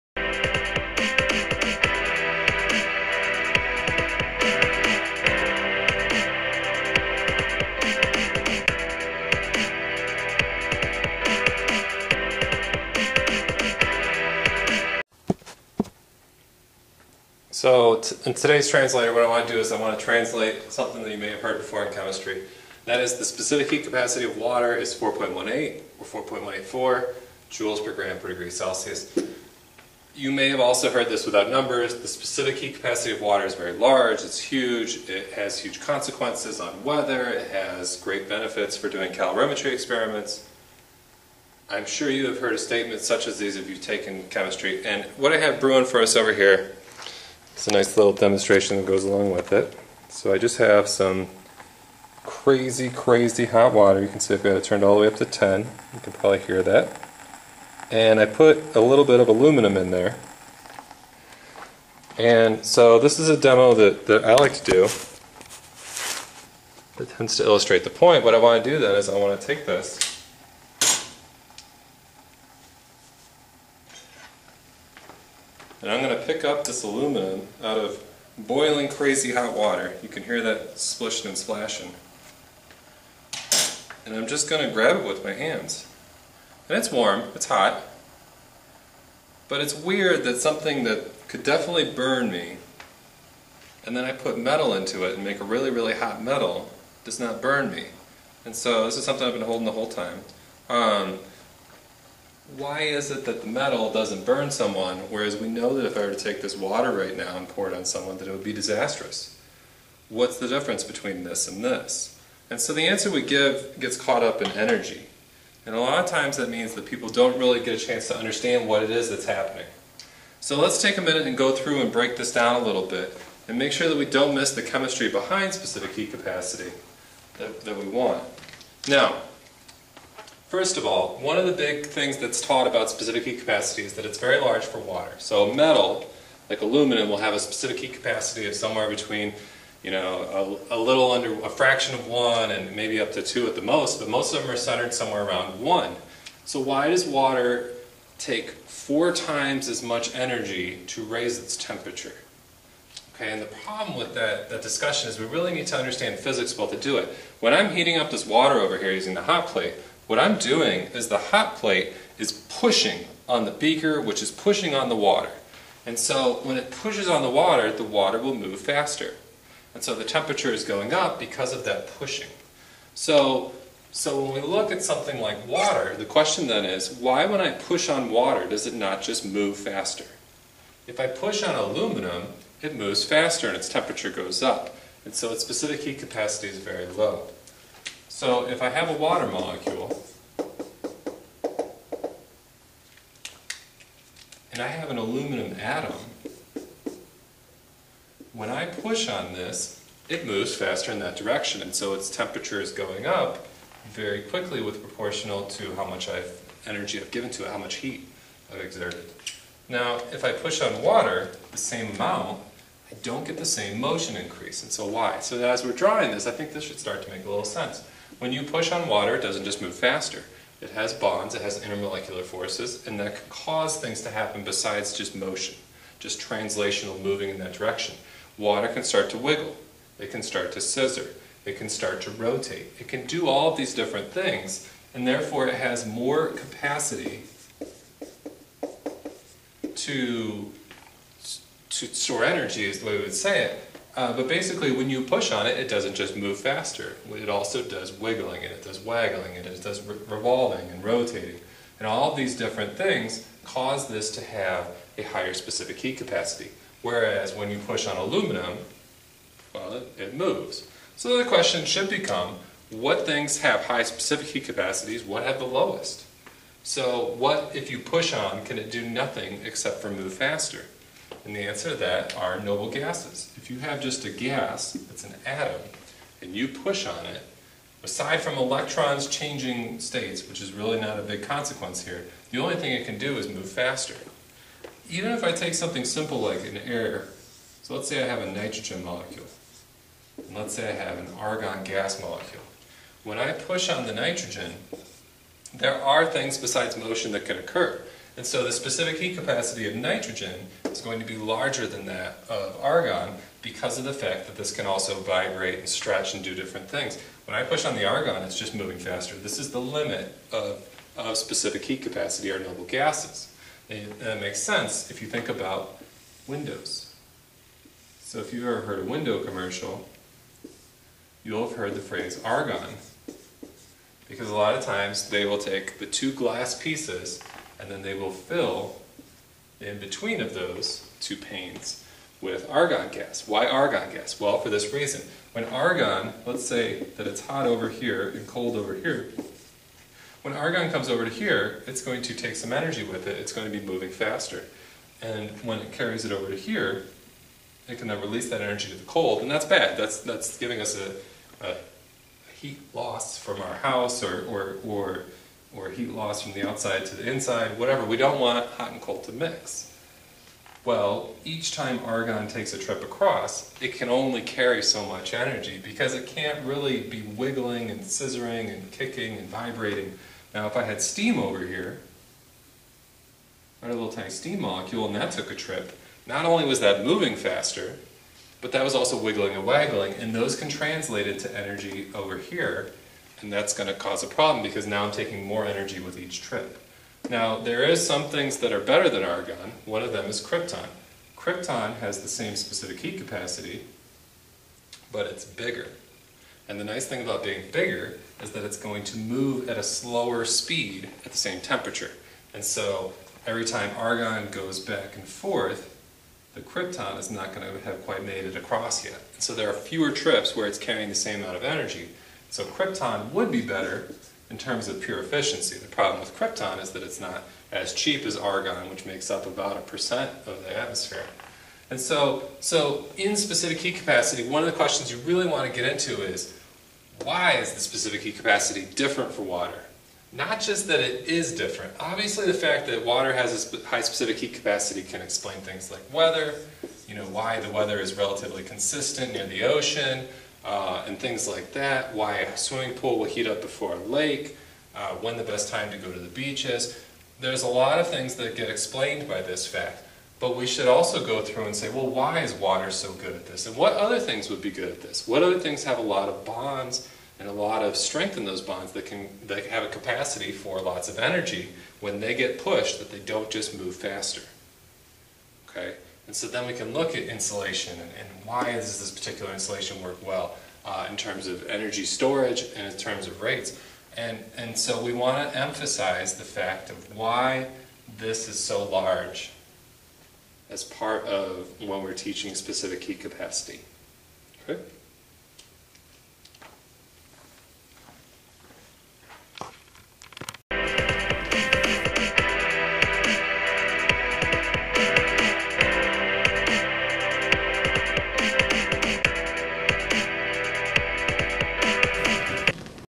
Chemistry Translator 4.184 specific heat of water
Find other chemistry sayings translated into more depth and understanding
This translation focuses on why water has a specific heat capacity instead of focusing on the consequences of the fact that water does have a high specific heat capacity.  Look at what happens when you push water molecules that is different from pushing on aluminum atoms.